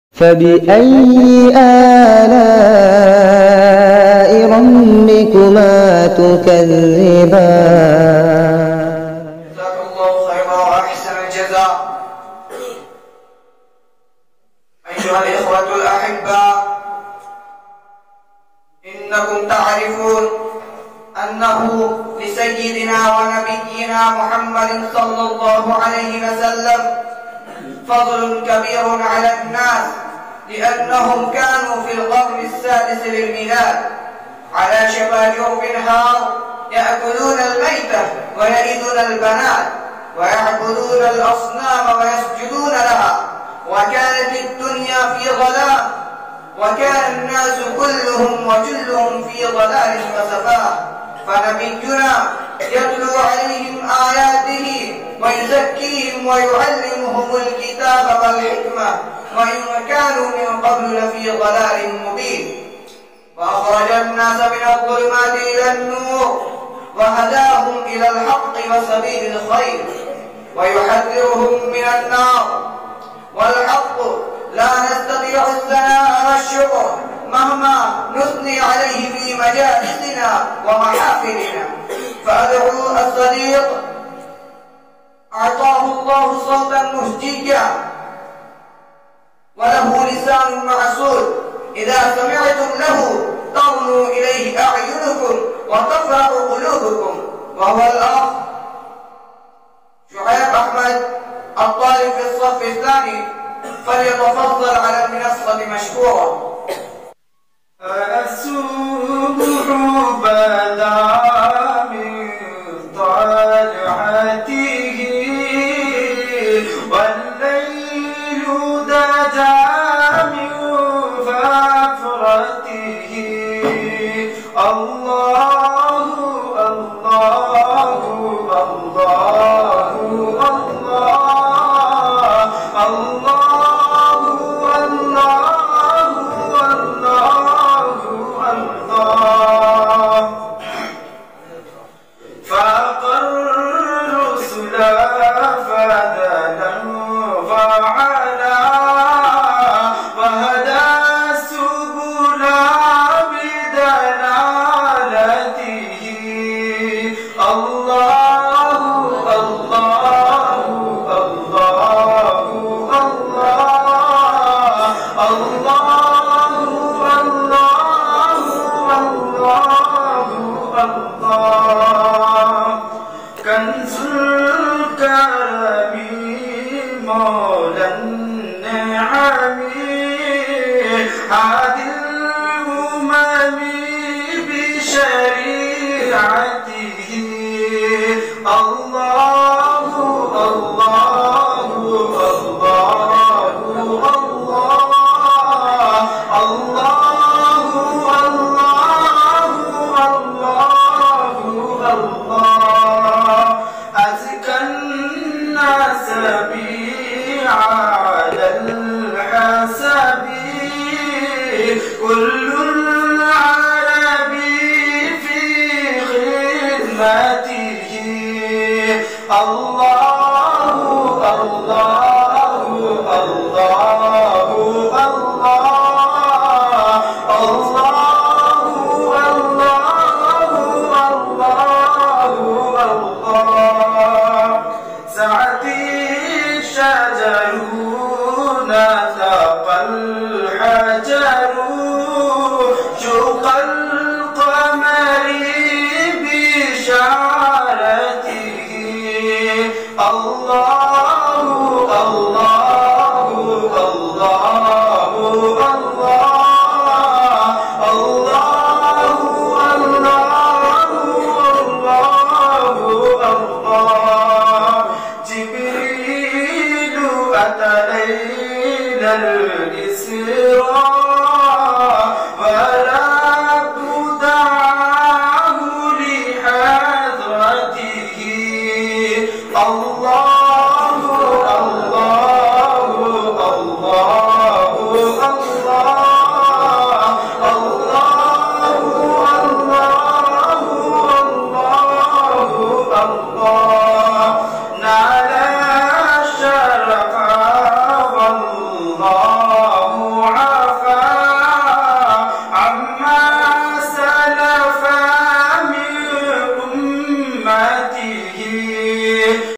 مدح النبي صلي الله عليه وسلم / الاحتفال النهائي للنادي الأدبي لطلبة مدرسة تحفيظ القرآن بتنة
مدح النبی صلی اللہ علیہ وسلم